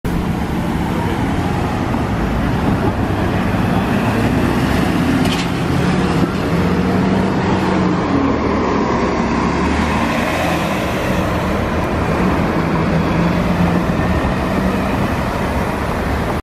CTtransit #1866 on Route 243B
The last of the XD40s...for now.

2017 New Flyer Xcelsior XD40 #1866 operating on Route 243B to Brookside Housing Complex via SCSU and Whalley Avenue.

Cummins L9
Allison B400R-6